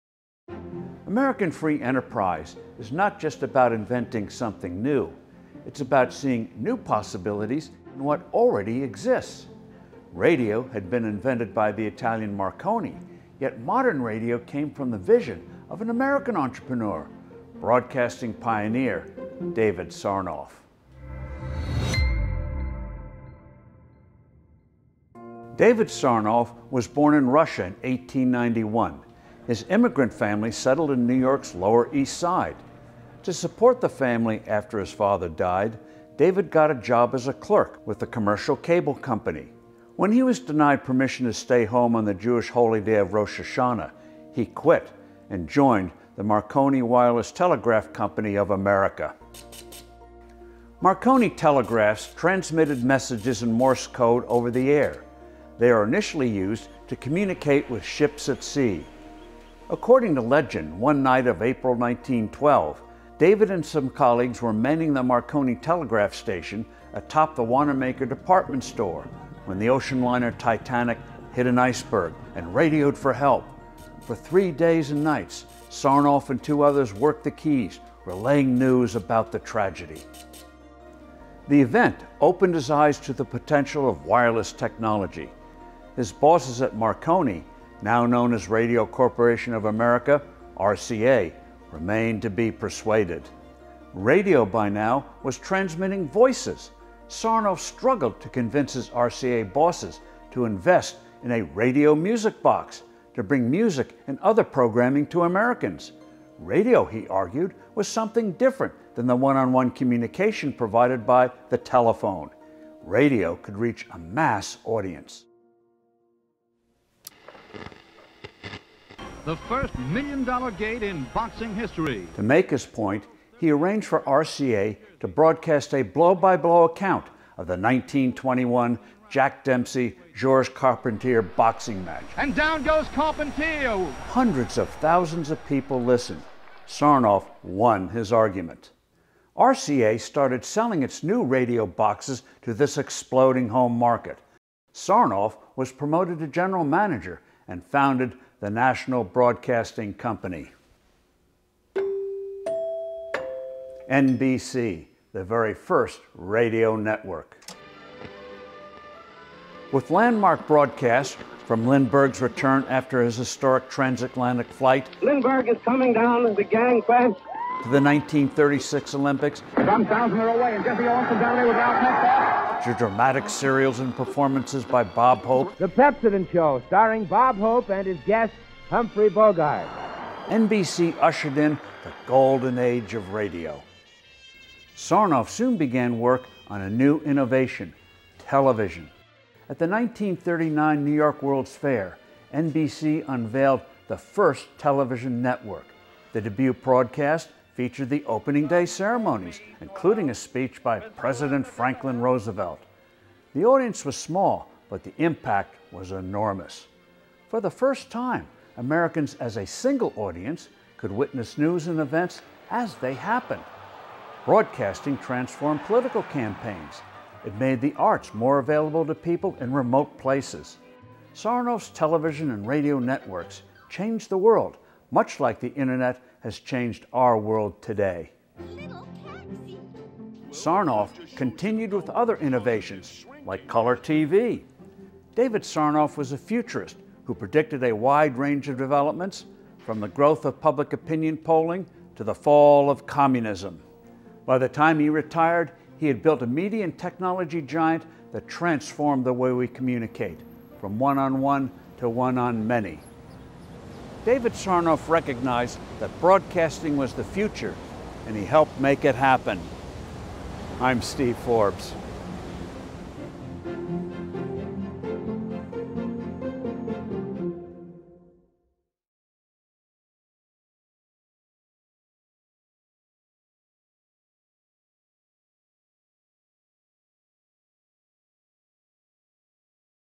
The immigrant who forever changed news and entertainment.
Steve Forbes on radio and television pioneer David Sarnoff, who recognized the powerful potential of broadcasting to a mass audience.

Producer/Writer/Series Creator – Elizabeth Ames
Producers – Kip Perry, Elan Bentov, Thomas Skinner, Robert Chatfield
Director - Kip Perry
Editor – Erik Swanson